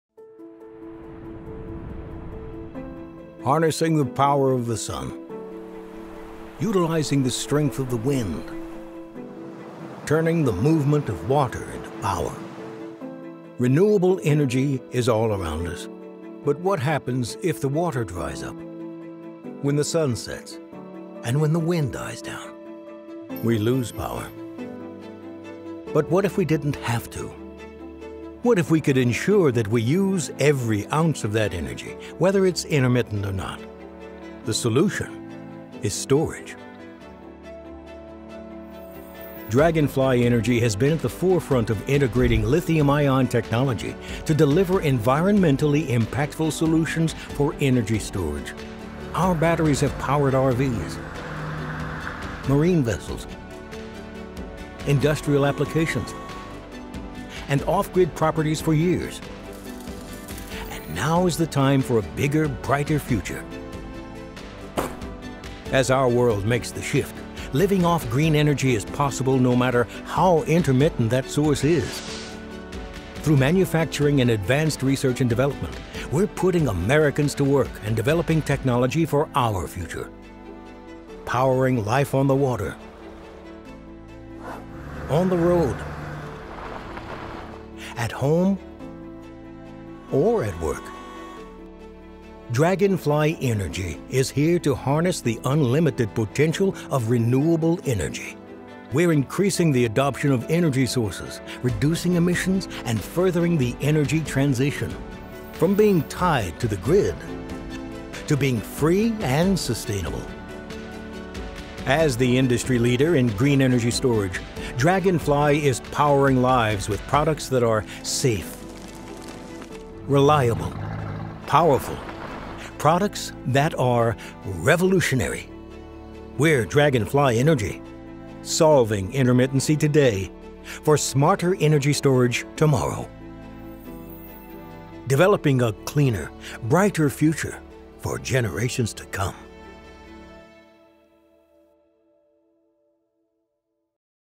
At the Forefront of Lithium-Ion Technology for Renewable Energy Storage | We're Dragonfly Energy
Dragonfly Energy is the leader in lithium-ion deep cycle batteries, making renewable energy accessible and affordable. Well-known for the consumer brand, @battlebornbatteries, our revolutionary, non-toxic line of products are made to be drop-in replacements for lead acids batteries in RVs, marine vessels, material handling, off-grid applications, and more. Dragonfly Energy is leading the charge in green energy storage.